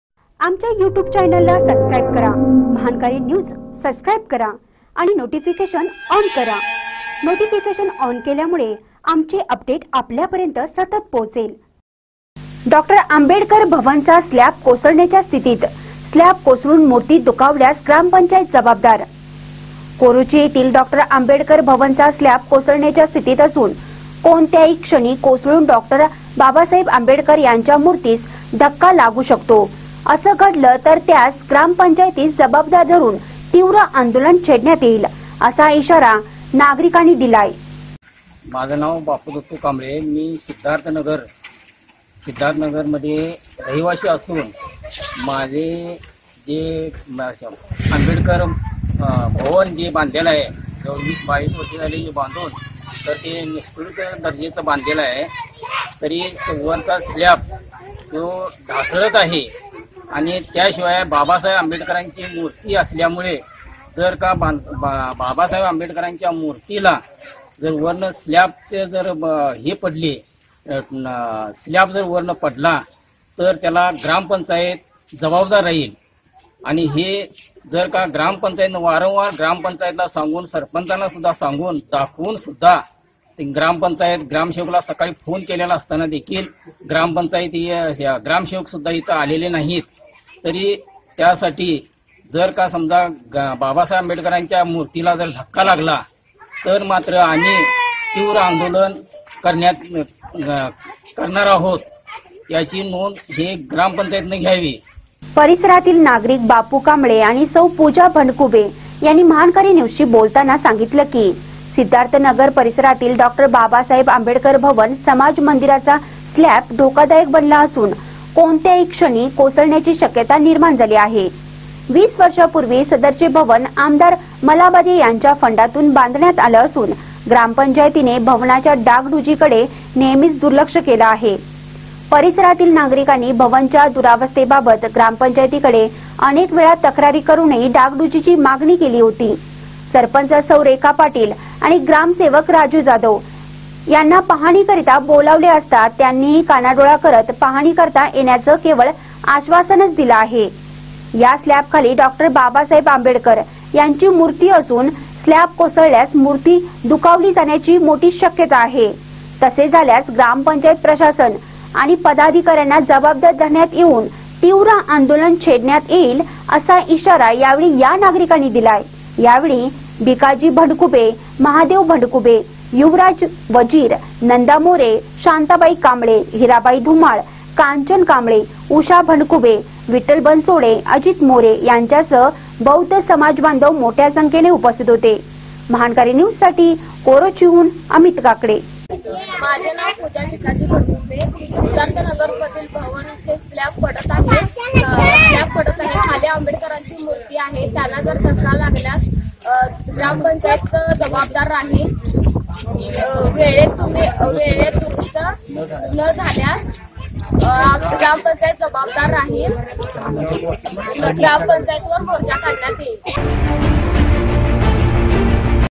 डॉ.आंबेडकर भवनचा स्लॅब कोसळण्याच्या स्थितीत
डॉ.आंबेडकर भवनचा स्लॅब कोसळण्याच्या स्थितीत : स्लॅब कोसळून मूर्ती दुखावल्यास ग्रामपंचायत जबाबदार